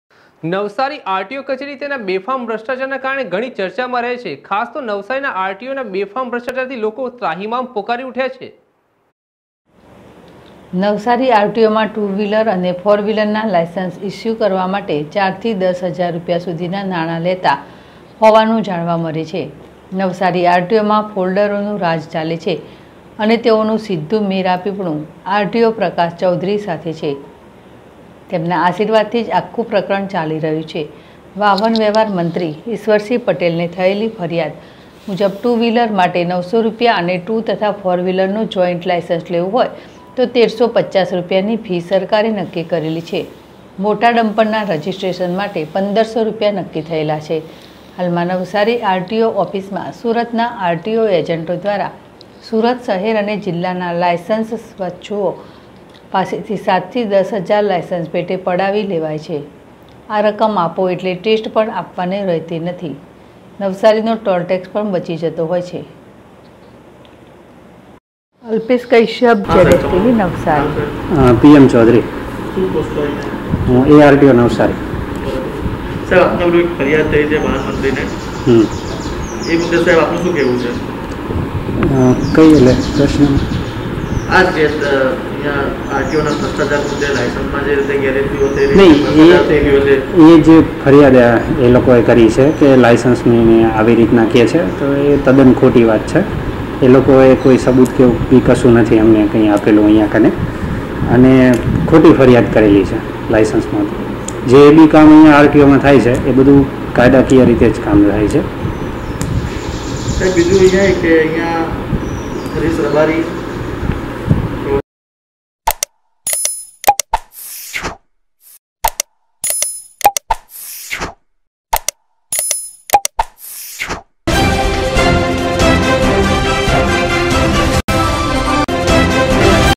નવસારી આરટીઓ કચેરી તેના બેફામ ભ્રષ્ટાચારને કારણે ઘણી ચર્ચામાં રહે છે
નવસારી આરટીઓ કચેરી તેના બેફામ ભ્રષ્ટાચારને કારણે ઘણી ચર્ચામાં રહે છે ખાસ તો નવસારીના એઆરટીઓનો બેફામ ભ્રષ્ટાચારથી લોકો ત્રાહિમામ પોકારી ઉઠ્યા છે//ZSTVNEWS//.